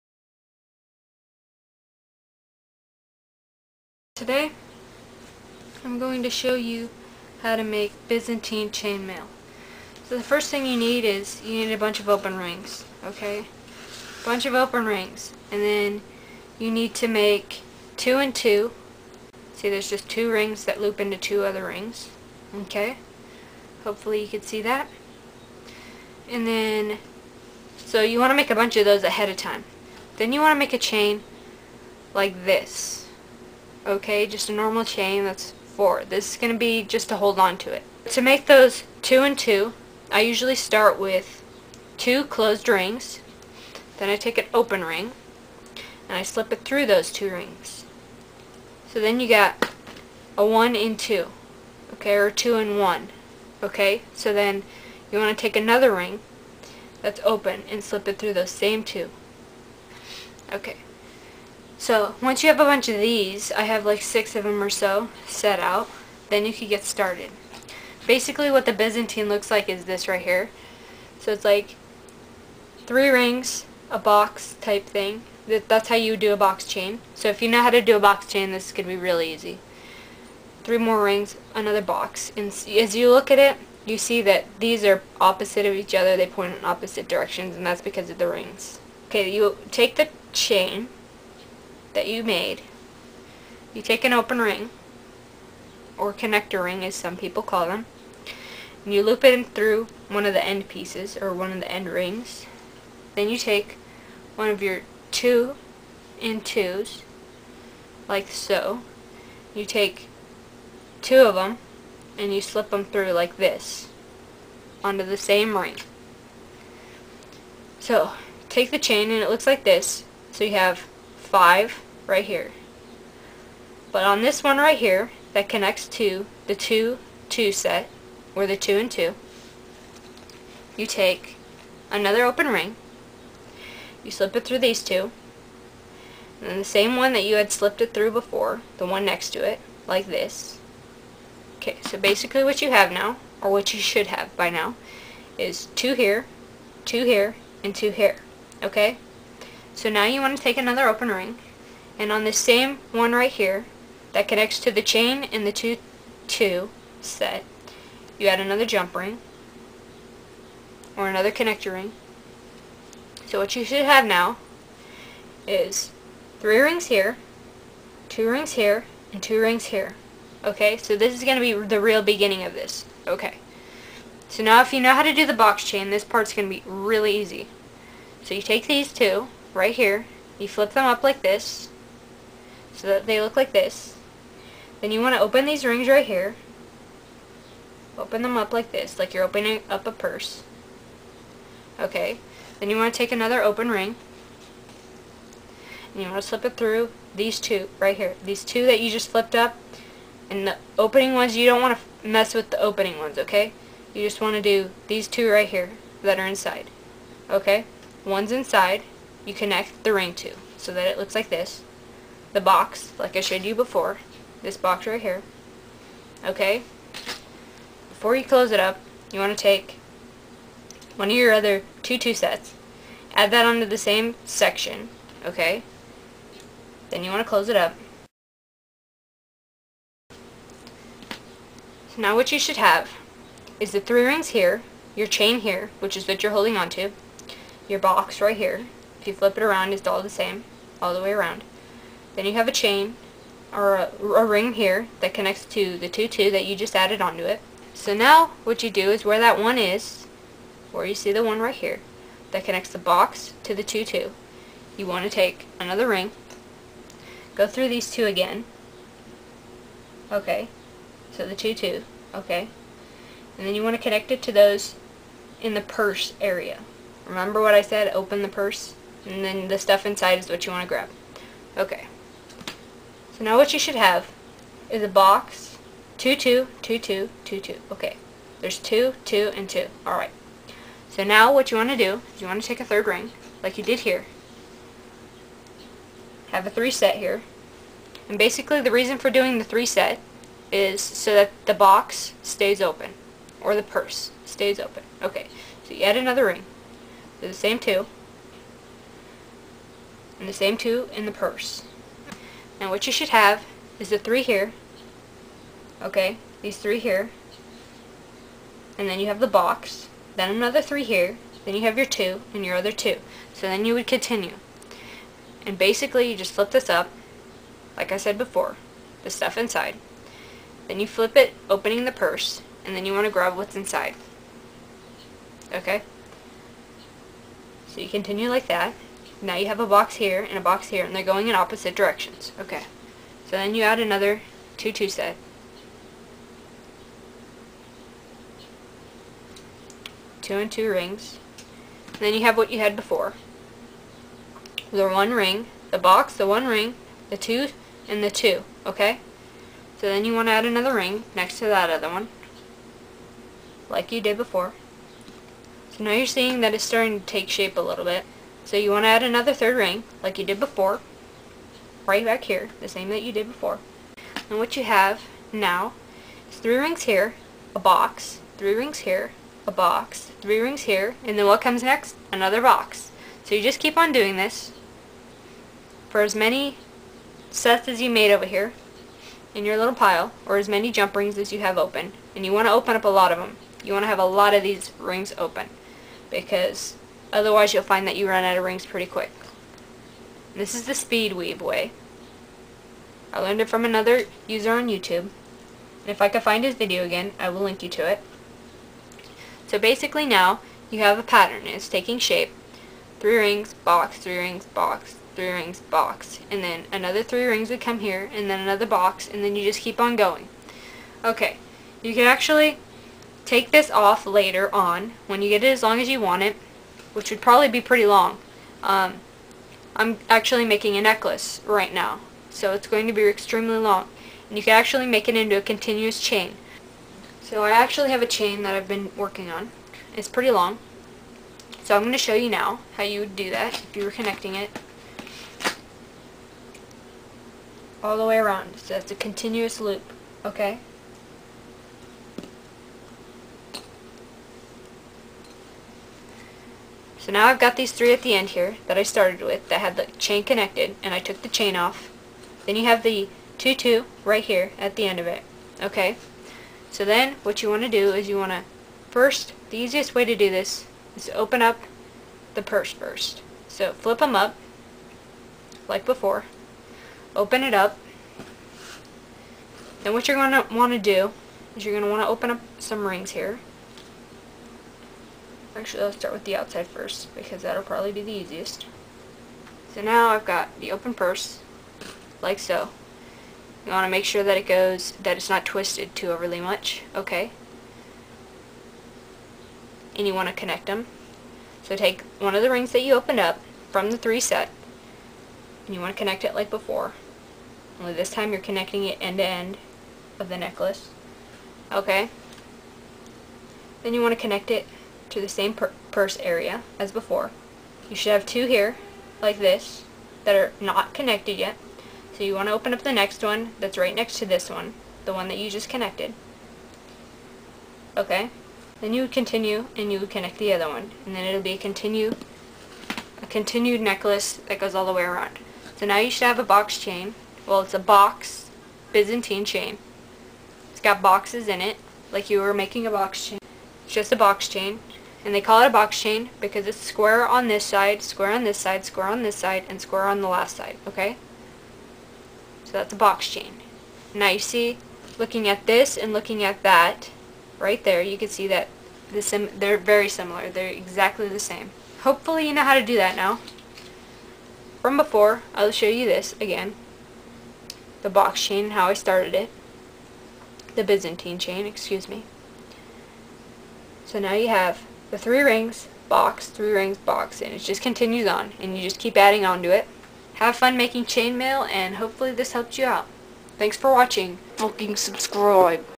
Byzantine Chainmail
A guide to making Byzantine Chainmail.